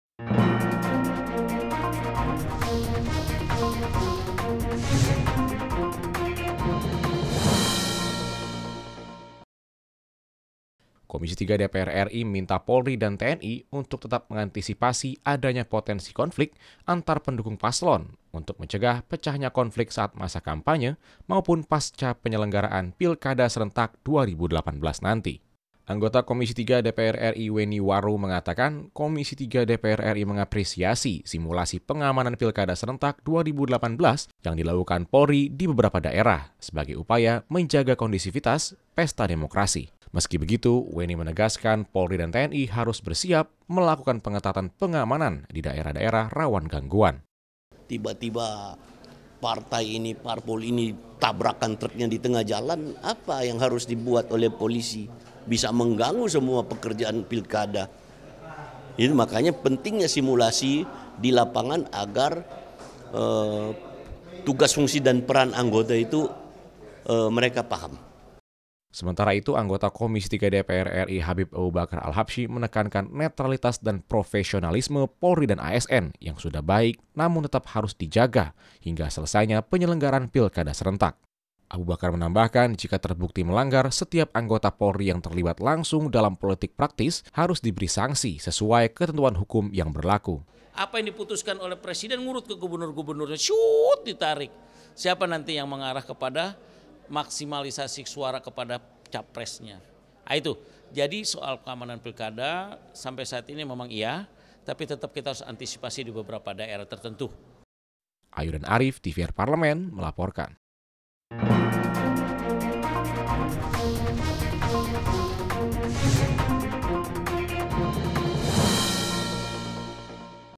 DPR RI - KOMISI III DPR RI MINTA POLRI TETAP ANTISIPASI PENANGANAN KONFLIK PILKADA
NARSUM : WENNY WAROUW / Anggota Komisi III DPR RI / FPGERINDRA / Dapil SULUT
HABIB ABOE BAKAR ALHABSYI / Anggota Komisi III DPR RI / FPKS / Dapil KALSEL I